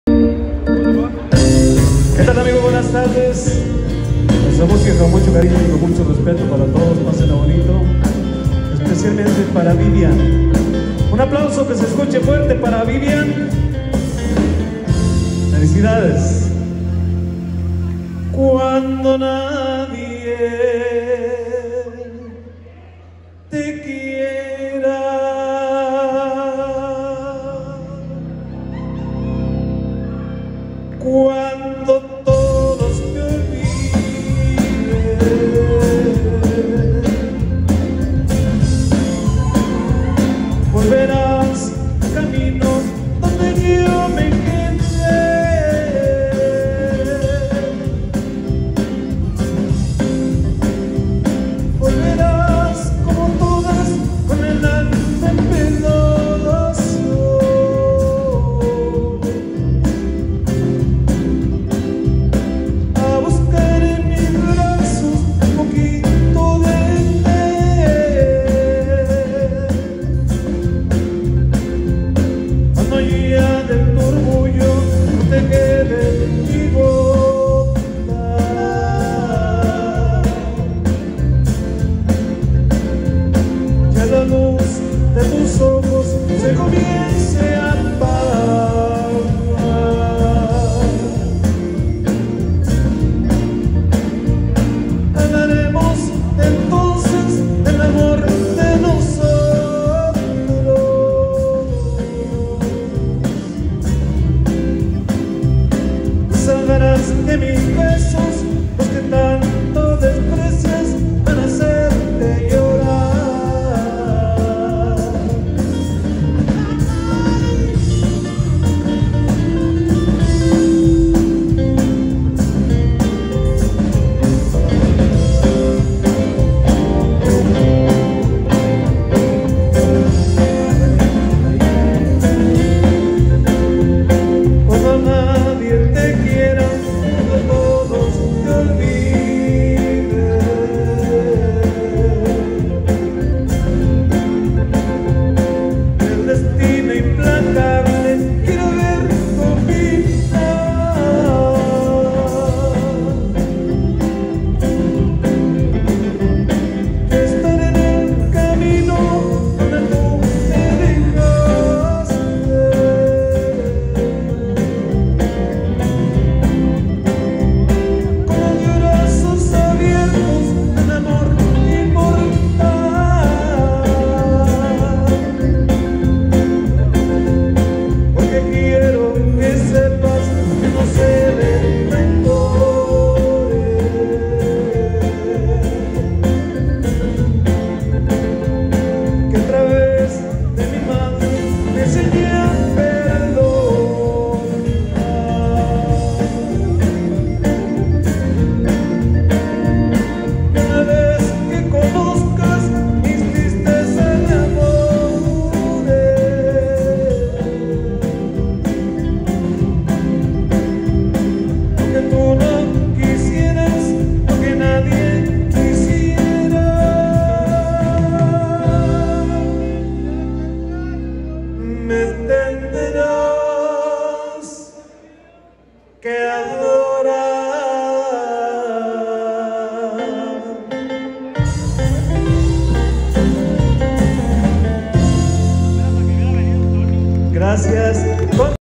Los Barón de Apodaca-Pueblo Nuevo Gto.
Los Baron de Apodaca festejando los XV años de Vivían Espitia Espitia.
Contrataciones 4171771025